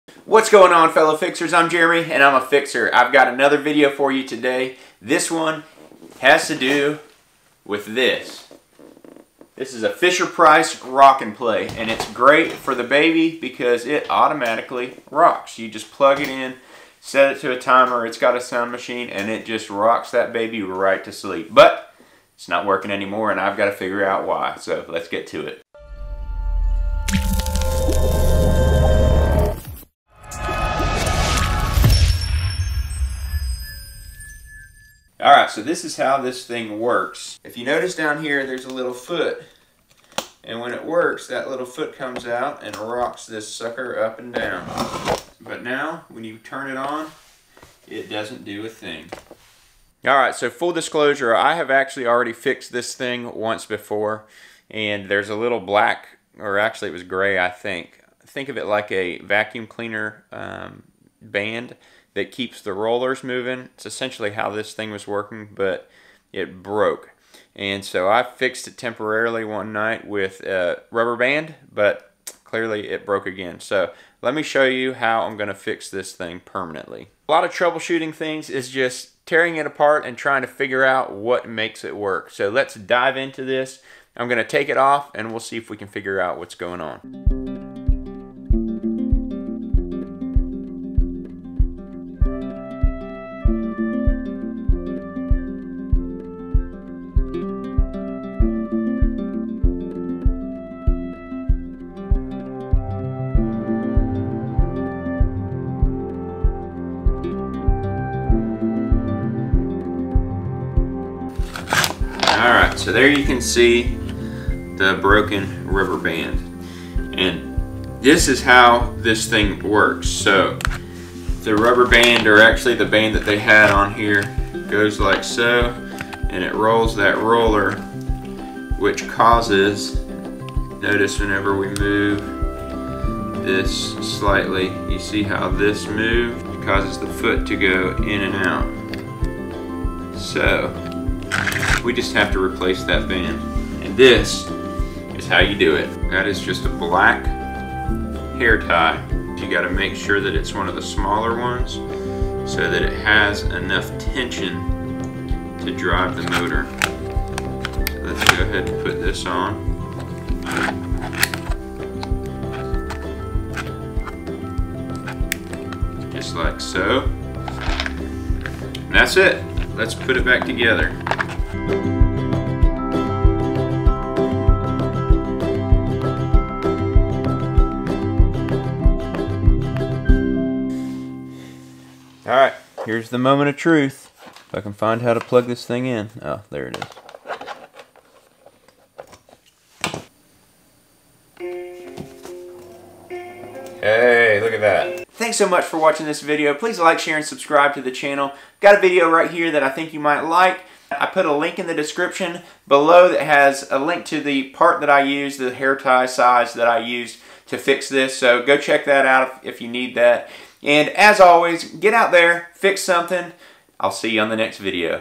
Fix Rock n Play
Our Rock n Play stopped rocking, but I'm going to show you how to fix it.

This fisher price rock n play is one of the best purchases we made for the baby. It made it so easy to put the baby to sleep.

The problem is that after using it with two kids, the band that makes the foot rock broke.

It's an easy and inexpensive fix, and I'm going to show you how to do it.

If you need want one of these, or need the part to fix it, here they are:
Fisher Price Rock n Play